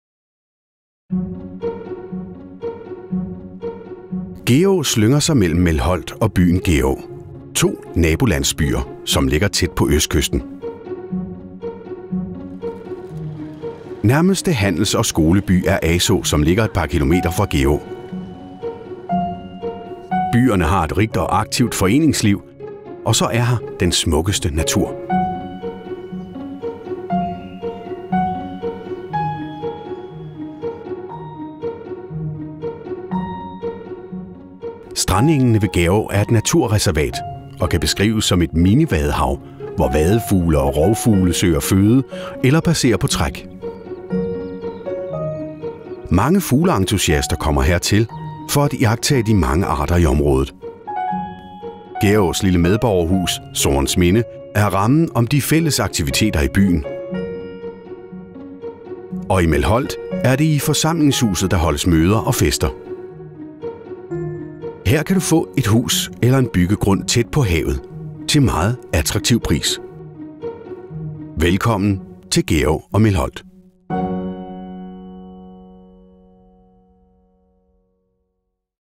Velkommen til Geraa og Melholt
Gerå slynger sig mellem Melholt og byen Gerå - to nabolandsbyer som ligger tæt på østkysten.

Ved Grenaa findes Strandengene - et naturreservat, med mini vadehav, vadefugle og rovfugle, som mange fugle-entusiaster tager til.

Nærmeste handels - og skoleby er Asaa som ligger et par kilometer fra Geraa.

Byerne har et rigt og aktivt foreningsliv og den smukkeste natur.
Geraas lille medborgerhus Sorensminde og Forsamlingshuset i Melholt danner rammerne for de fælles aktiviteter, møder og fester.

Her kan du få et hus eller en byggegrund tæt på havet til en meget attraktiv pris.